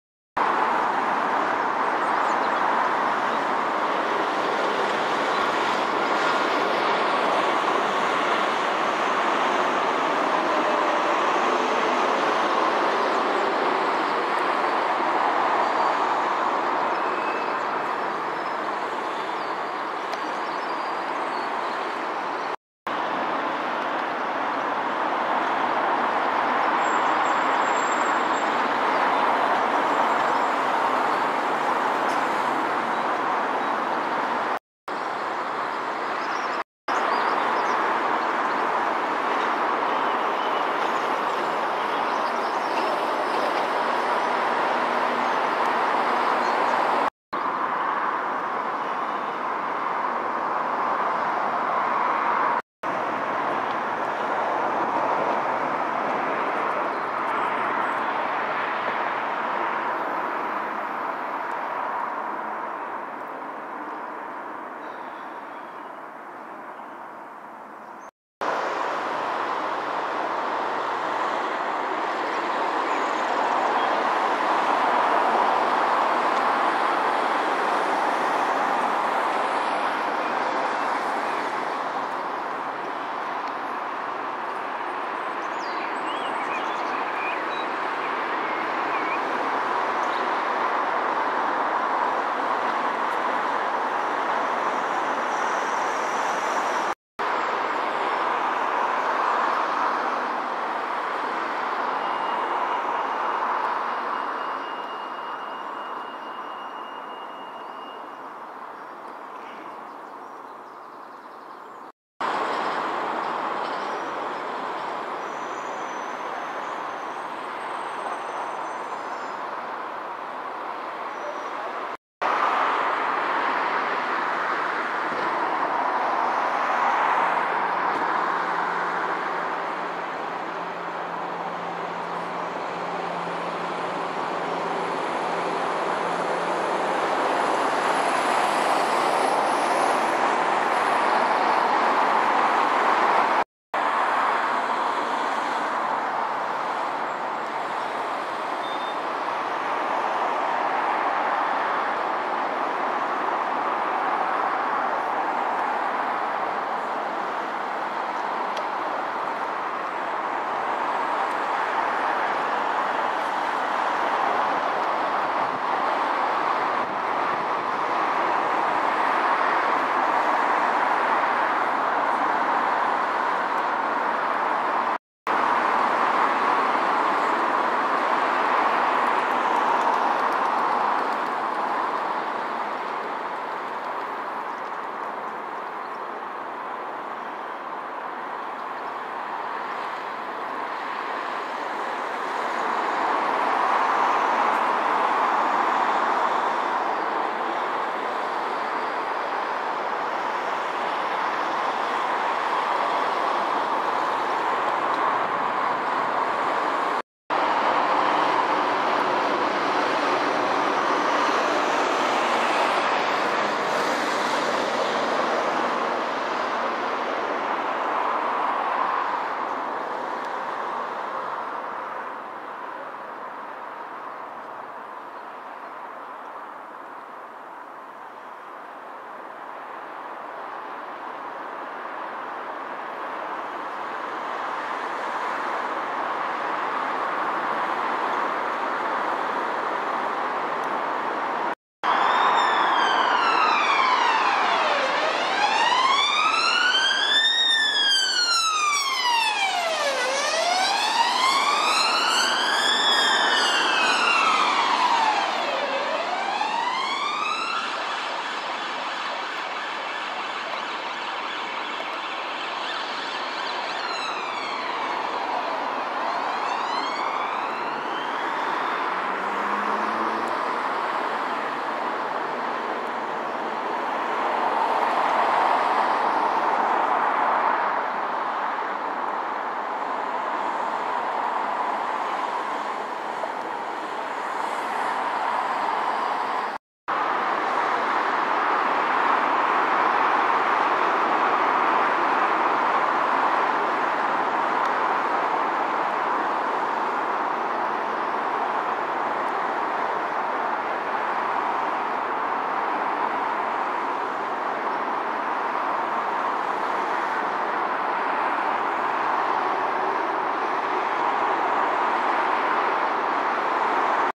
oct-18 lenses
I could have done with a tripod, and a focus puller, but here are some oct-18 35mm cine lenses mounted on a GH1, no lut or CC applied this is straight out of the camera.
lomo 18mm
lomo 29mm
kmz 28mm
kmz 35mm
lomo 50mm
kmz 75mm
jupiter 135mm